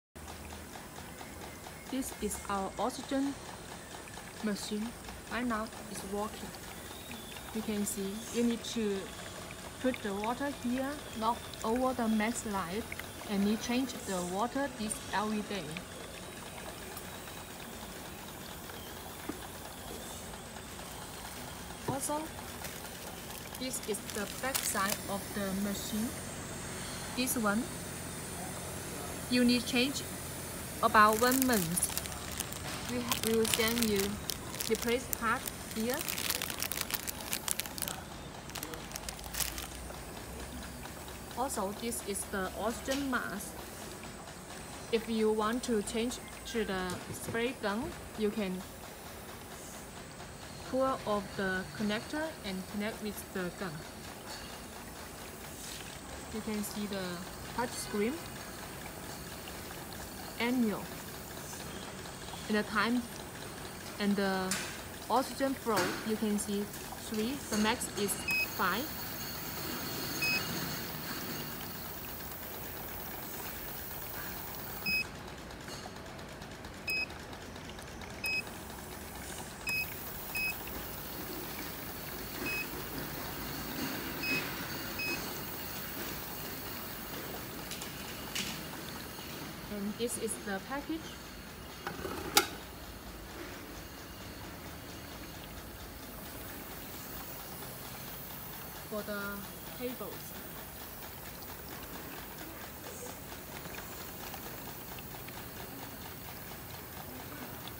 Korea O2toDerm oxygendome
O2toDerm hyperbaric anion oxygen system

The one of a kind dome generates the amount of anion ( negative anion) by 3 million more than natural production. Anions are vitamins from the air, similar to what you get in a deep forest. They induce and stabilized free radicals which in turn regenerations new cells, reduces bacteria, rebalances PH levels, improves signs of aging, corrects and heals damaged cells. In conjunction with the O2to Derm products it improves tone, texture and will instantly leave a lasting healthy glow.